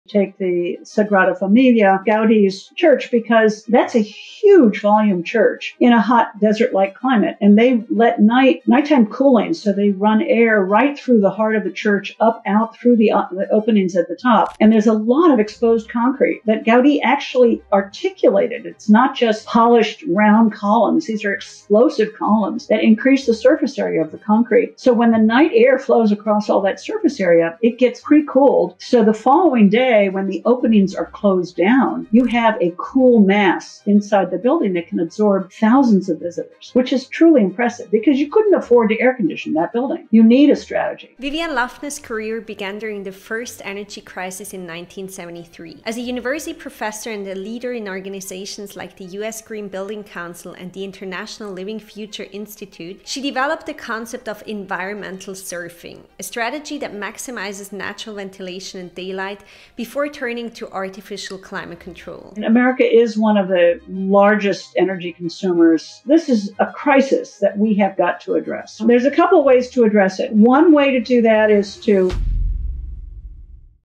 Why Modern Buildings Are Suffocating Us
Did you know buildings used to be designed to survive power outages… but now most can’t last a day without AC? 🫠

I just had the most fascinating conversation with Vivian Loftness, who has spent 50 years ho researching how to make buildings healthier, smarter, and more resilient. And let me tell you - her insights blew me away.

She started her career during the first energy crisis, studying passive solar design and climate-adaptive buildings (long before sustainability was a buzzword). While studying in Finland, she saw firsthand how architects prioritize daylight, natural ventilation, and material efficiency - things we should be doing everywhere.

Now, as a professor at Carnegie Mellon and a leader in the US Green Building Council, Vivian is pushing for buildings that don’t rely on machines to function:

Imagine hospitals that cut cooling energy by ⅔ with natural ventilation and rooftop gardens that actually feed patients. Or homes designed to stay livable even if the power goes out.

If you care about sustainability, design, or just living in a healthier space, you need to hear this episode.

COMMENT "SAGRADA FAMILIA" and I'll send you the link to the episode 🌱